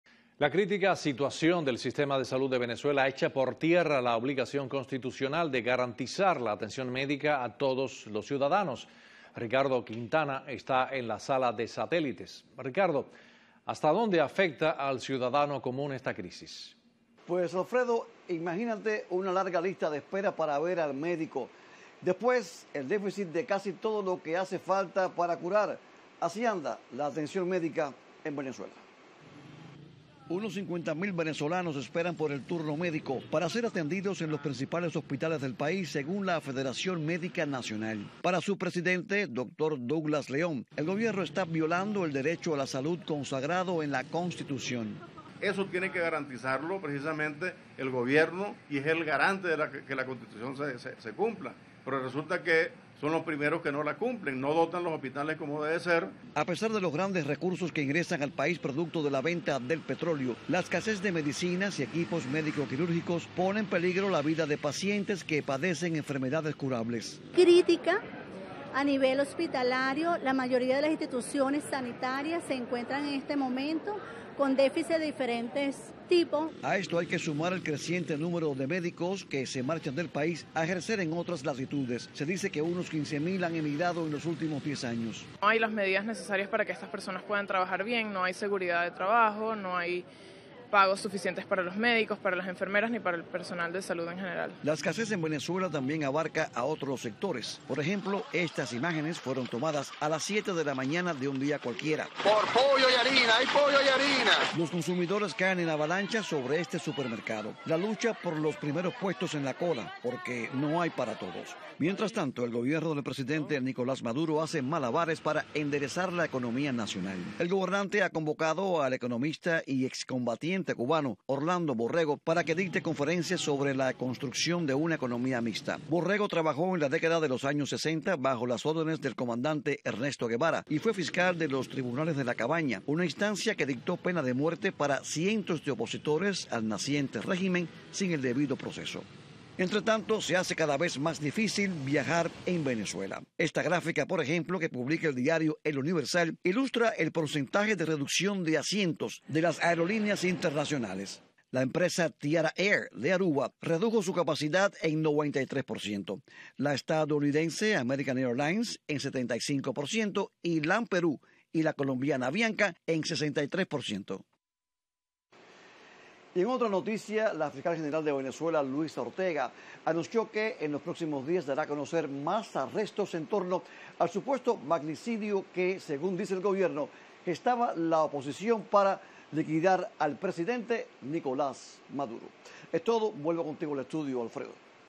Grave crisis de la salud pública en Venezuela
Junio 2| Aguda crisis en el sistema de salud pública en Venezuela, Donde es larga la espera Para Recibir atención médica y aguda la carencia de equipos y medicinas. Ricardo Quintana informa.